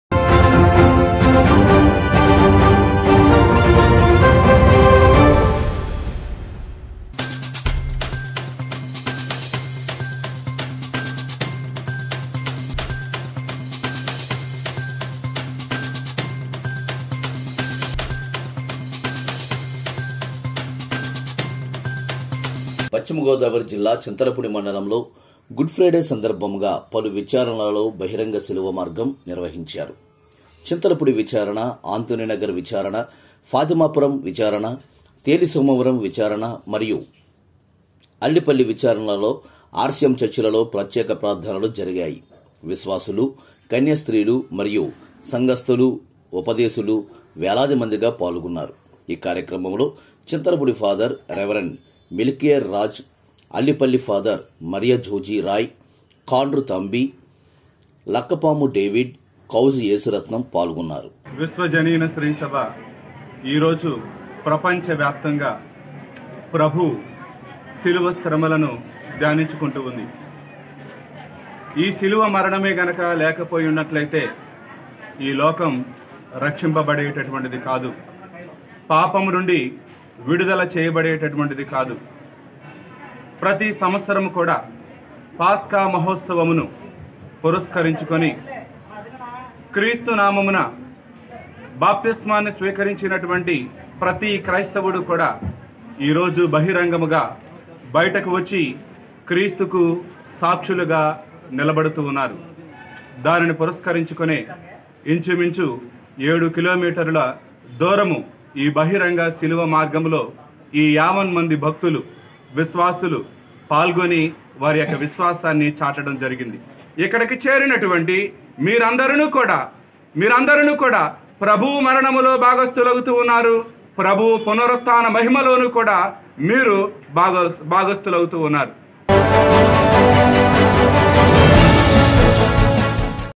NN1 NEWS:పశ్చిమగోదావరి జిల్లా.చింతలపూడిలో గుడ్ ఫ్రైడే సందర్భముగా పలు విచారణలలో ప్రత్యేక ప్రార్థనలు.
పశ్చిమగోదావరి జిల్లా.చింతలపూడిలో గుడ్ ఫ్రైడే సందర్భముగా పలు విచారణలలో ప్రత్యేక ప్రార్థనలు.